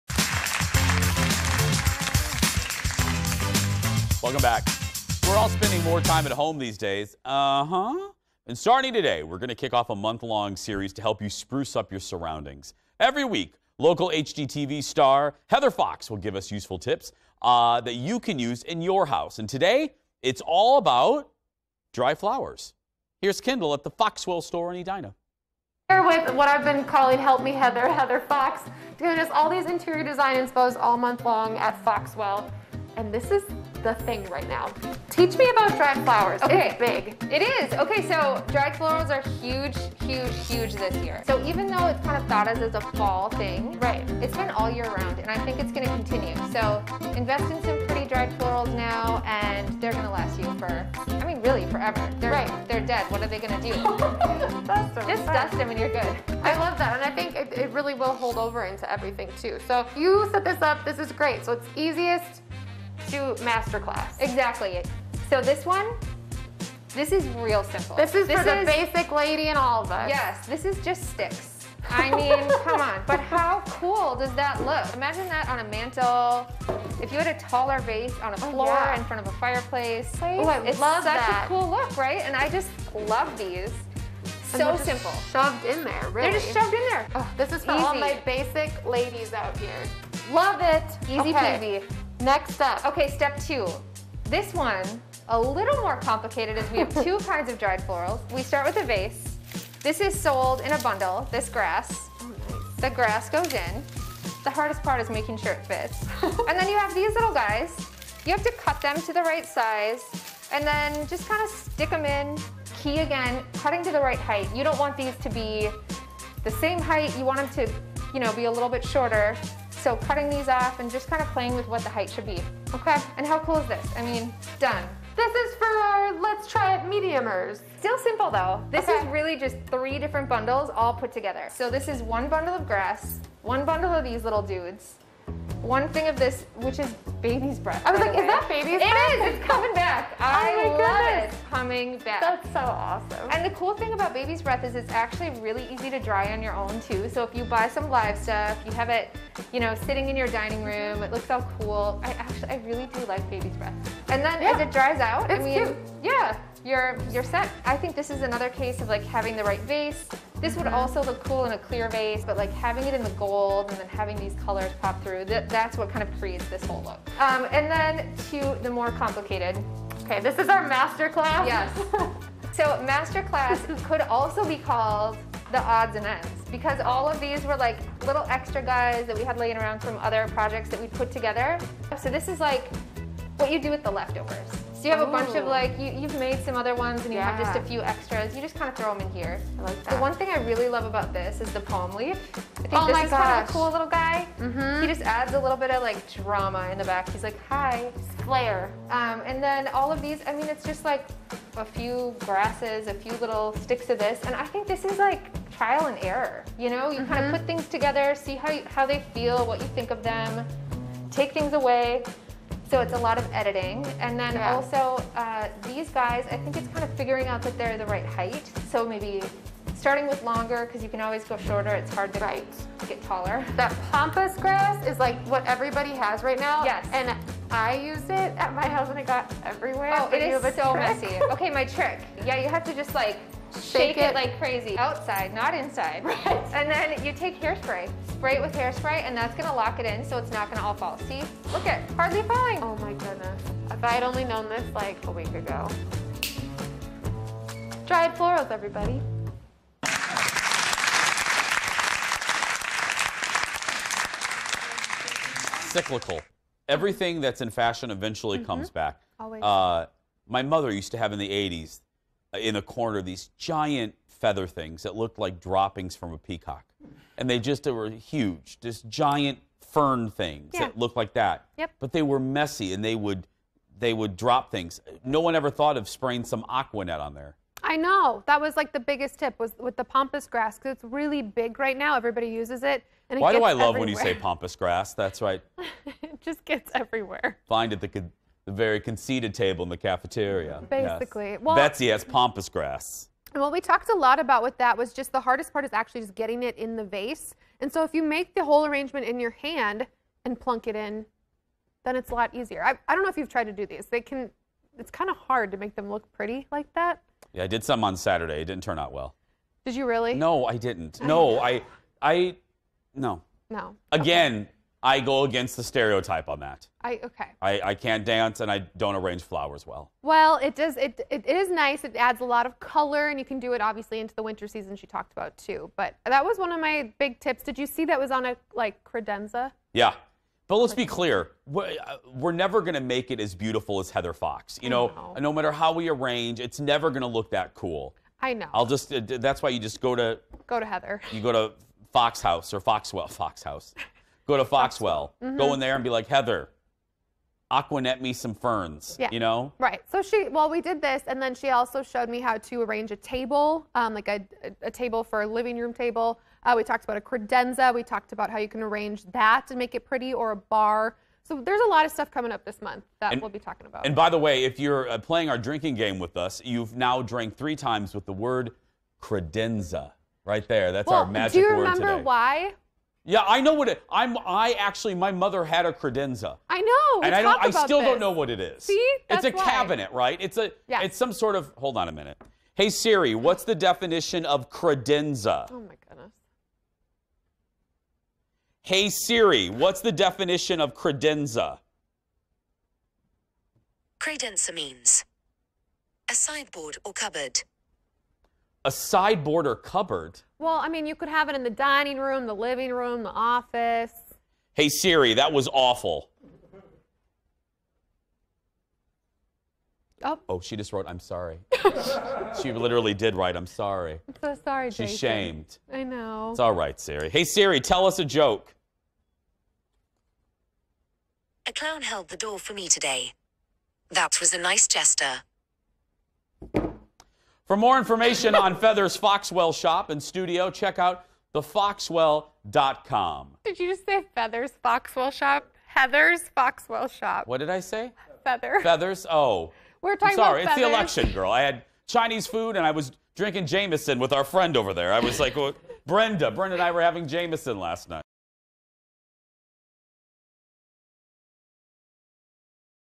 Interior design with Heather Fox
Kendall visited Foxwell to get some interior design tips with dried florals from Heather Fox.